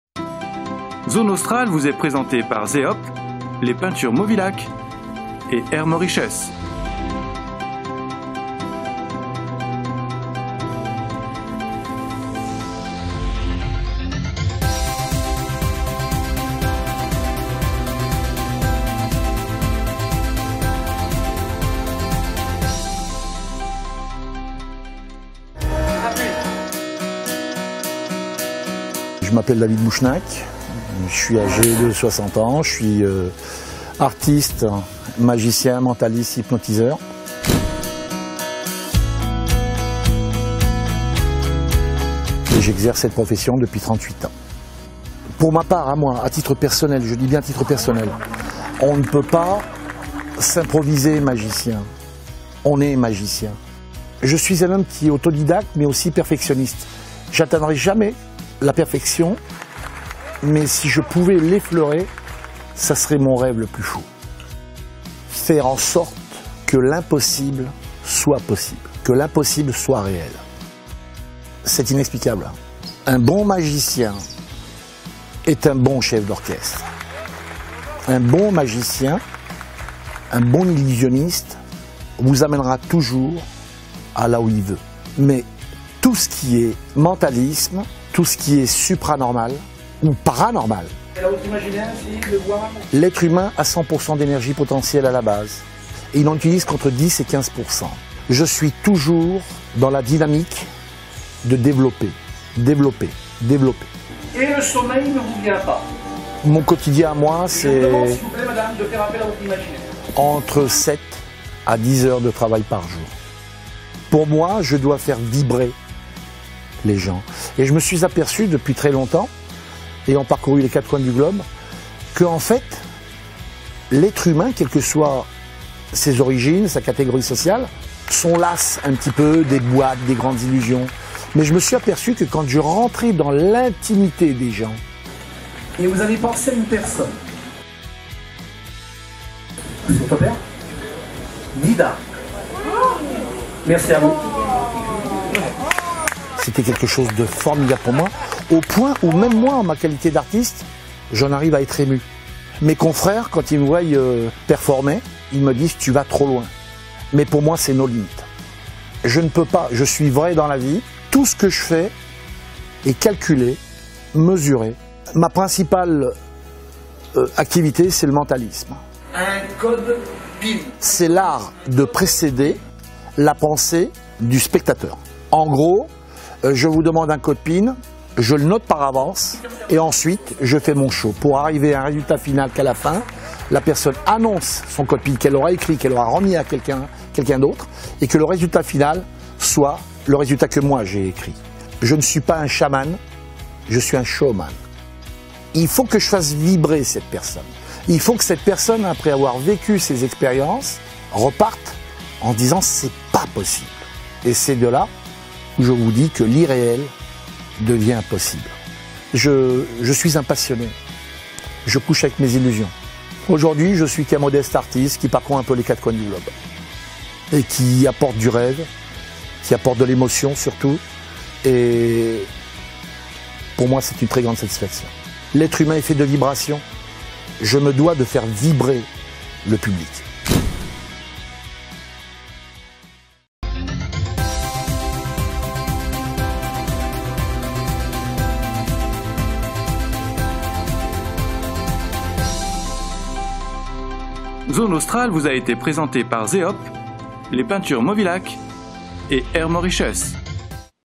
Film David Bouchenak Magicien, Mentaliste et Hypnotiseur
Portrait TV David Bouchenak Magicien, Mentaliste et Hypnotiseur, réalisation Serge Marizy